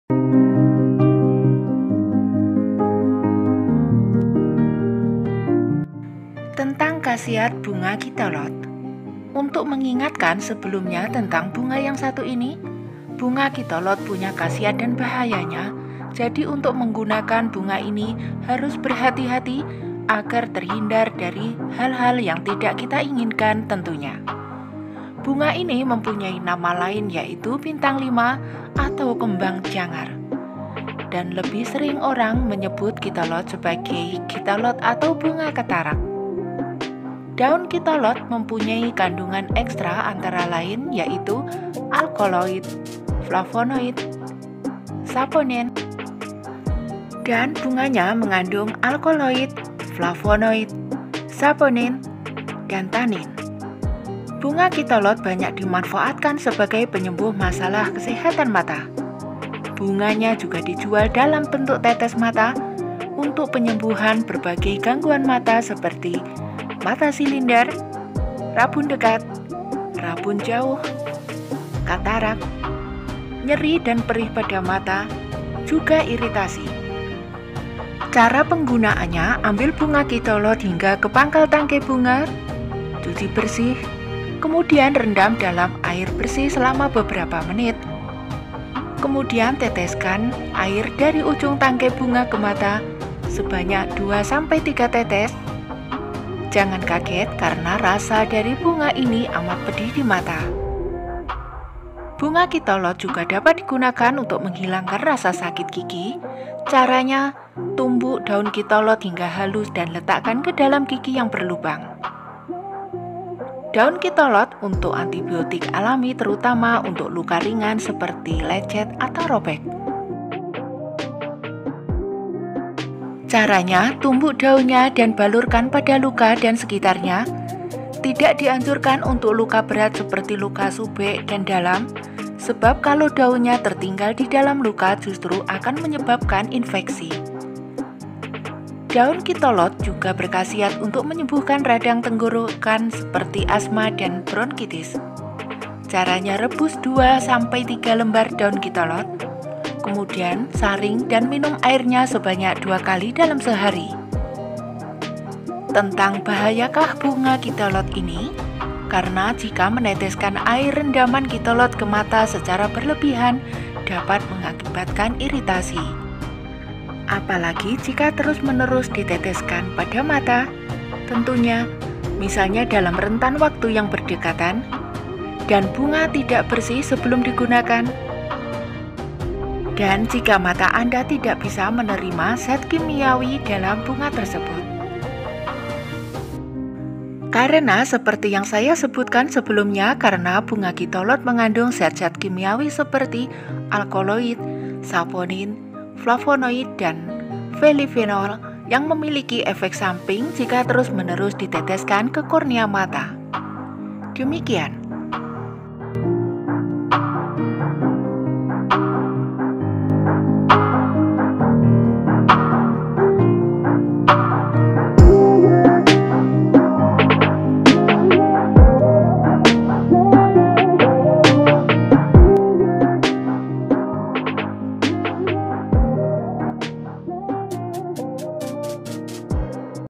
Manfaat Tanaman Kitolod
Deretan Manfaat Tanaman Kitolod, Salah Satunya Sebagai Antibiotik Alami - Fakta Menarik

Pernah mendengar tanaman kitolod? Tanaman berbatang lunak ini bisa mencapai ketinggian hingga 60 cm dan memiliki daun tunggal runcing dengan pangkal yang menyempit. Bunga kitolod mengandung senyawa saponin, alkaloid, polifenol, daun dan bunga kitolod yang ternyata memiliki banyak manfaat bagi kesehatan lho. Apa saja ya? Yuk kita cari tahu bersama!